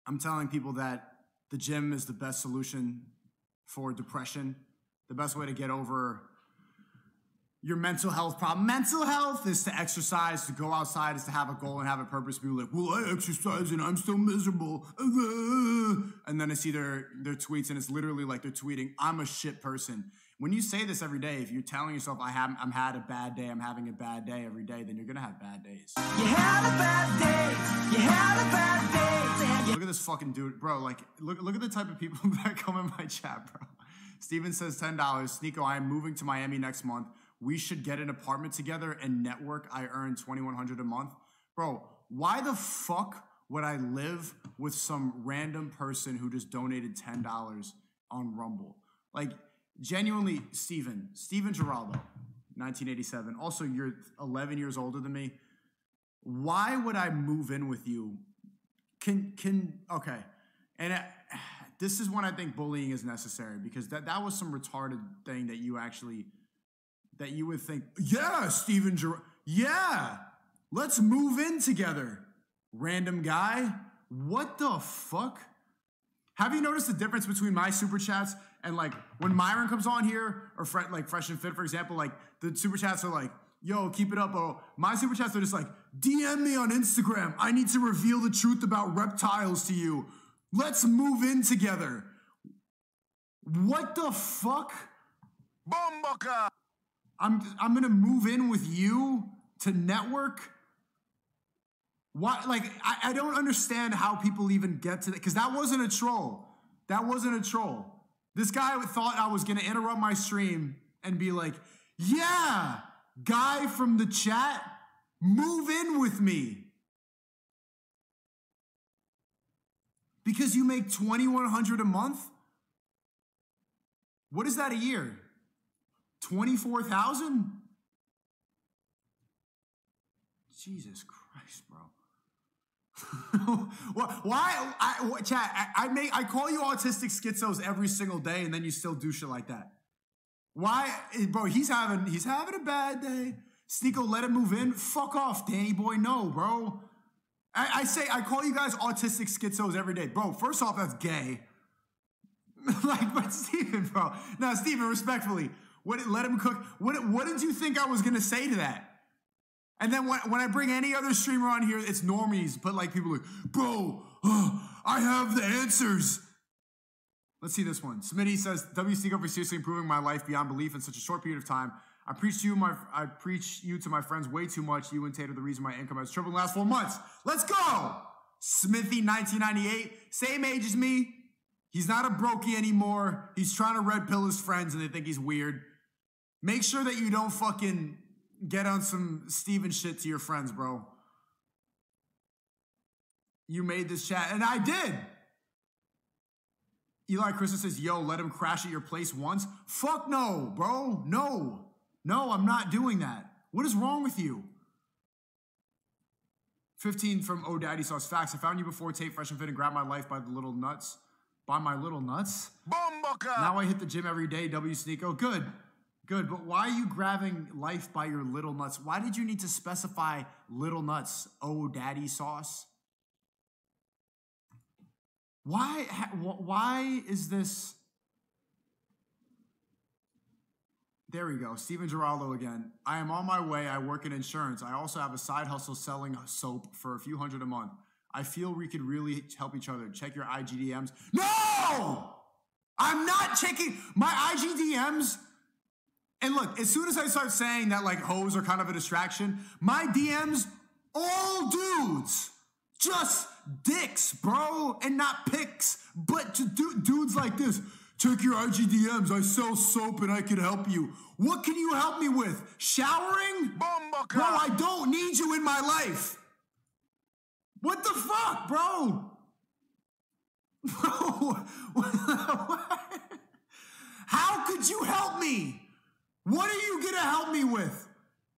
Sneako's Livestream Chat Gets SUS On Rumble!!
Sneako's Rumble Chat got real sus the other day! They were discussing red pill information and all of a sudden the Rumble chat got reeeaaal sussy. Do you guys enjoy the Sneako content?